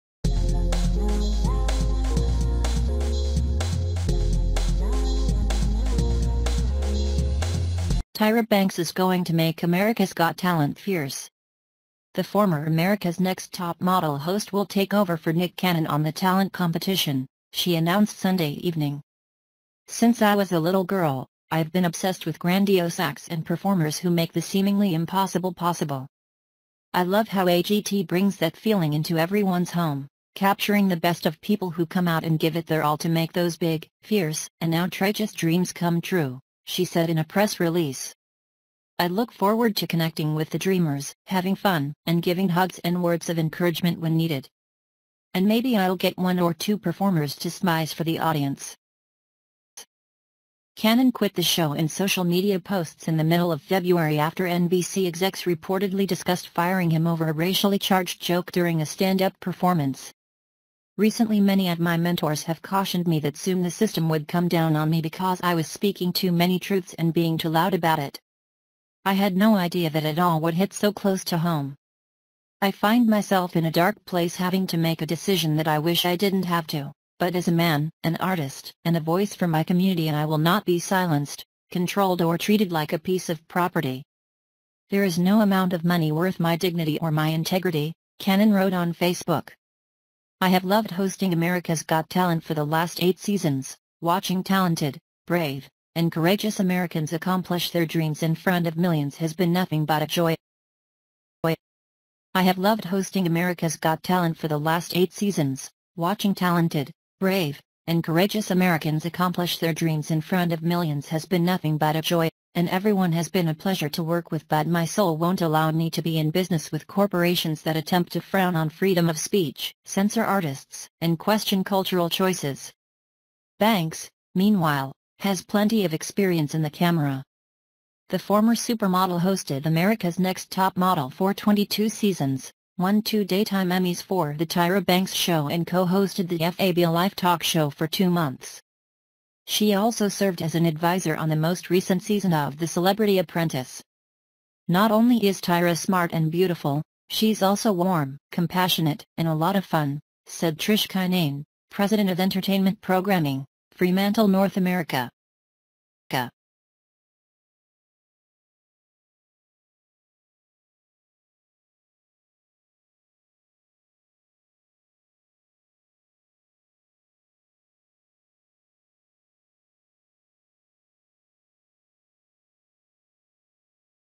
America’s Got Talent 2017: You think don’t know about it?
Tyra Banks will make "America's Got Talent" furious.

The previous "America's Next Top Model" host will assume control for Nick Cannon on the ability rivalry, she declared Sunday evening.

"Since I was a young lady, I've been fixated on affected acts and entertainers who make the apparently incomprehensible conceivable. I adore how "AGT" brings that inclination into everybody's home, catching the best of individuals who turn out and give it their everything to make those enormous, wild and over the top dreams work out," she said in a public statement.

"I anticipate associating with the visionaries, having some good times and giving embraces and inspirational statements when required. Furthermore, perhaps I'll get maybe a couple entertainers to smize for the group of onlookers!"

Gun quit the show in online networking posts amidst February after NBC executives purportedly examined discharging him over a racially charged joke amid an outstanding execution. "As of late large portions of my tutors have advised me that soon 'The System' would descend on me since I was talking excessively numerous truths and being to boisterous about it. I had no clue that everything would hit so up close and personal. I wind up in a dull place making a choice that I wish I didn't need to, however as a man, a craftsman, and a voice for my group I won't be hushed, controlled or treated like a bit of property. There is no measure of cash worth my poise or my trustworthiness," Cannon composed on Facebook.

"I have adored facilitating 'America's Got Talent for the last 8 seasons, watching gifted, overcome, and gutsy Americans finish their fantasies before millions has been only a delight, and everybody has been a joy to work with however my spirit won't permit me to be good to go with companies that endeavor to dislike the right to speak freely, blue pencil specialists, and question social decisions."

Banks, in the interim, has a lot of involvement in the camera. The previous supermodel facilitated "America's Next Top Model" for 22 seasons, won two Daytime Emmys for "The Tyra Banks Show" and co-facilitated the "FABLife" syndicated program for two months. She likewise filled in as a consultant on the latest period of "The Celebrity Apprentice."

"Not exclusively is Tyra keen and lovely, she's likewise warm, empathetic and a great deal of fun," said Trish Kinane, President of Entertainment Programming, Fremantle North America. "She will fit directly into the insane universe of 'AGT.'"

Recording of season 12 of "America's Got Talent" was deferred after the demise of judge Mel B's. dad, however is relied upon to continue soon with extra judges Howie Mandel, Simon Cowell and Heidi Klum.